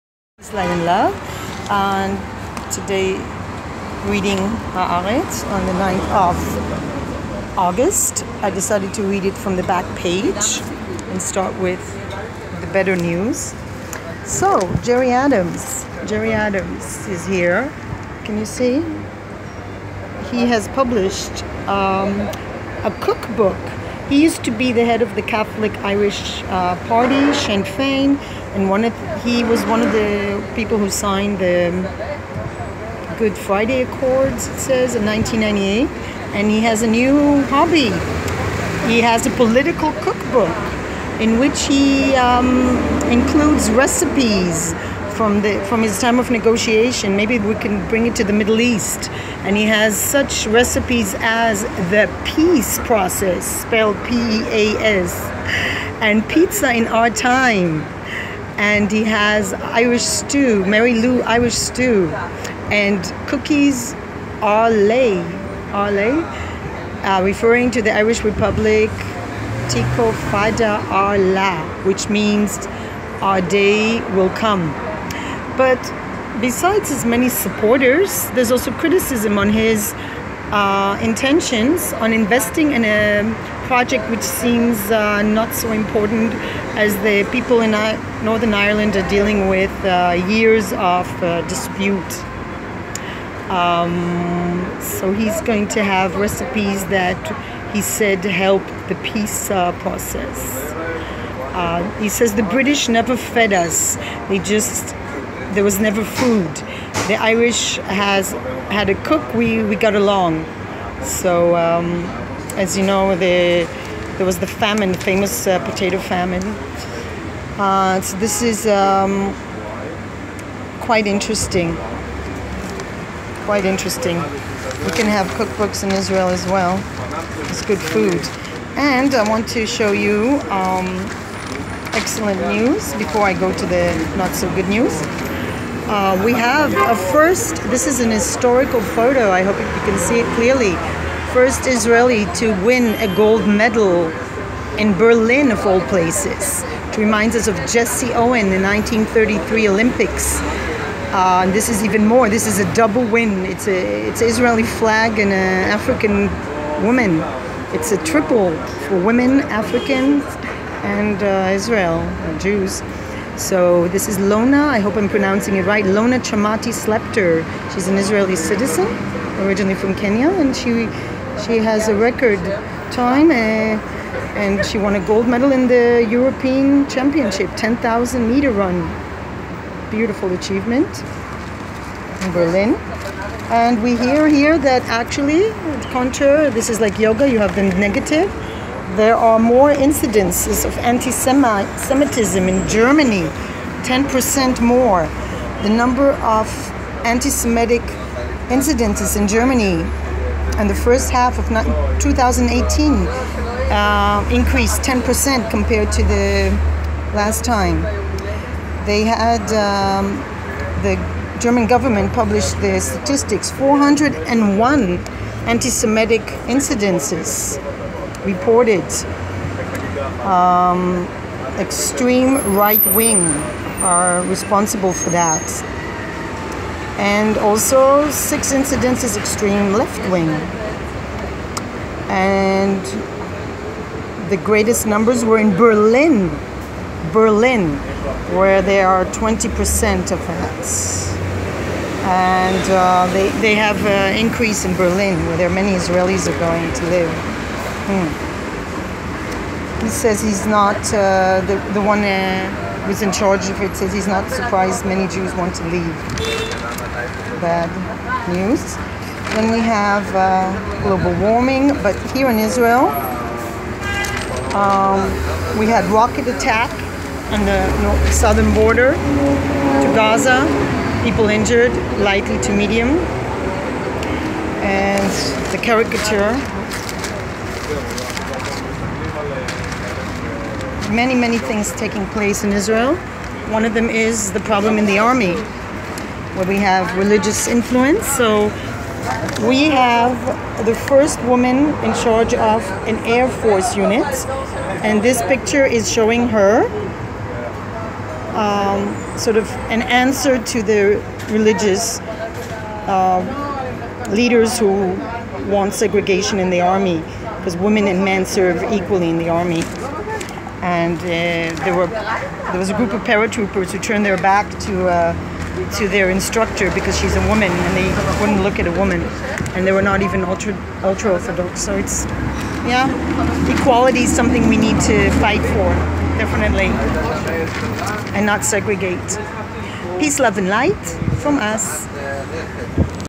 Left to Israel -9.8.18-the peas process
Critical reading of Haaretz:peace ,sports and anti-Semitism in Germany ,rocket attack in the South ,Segregation in the army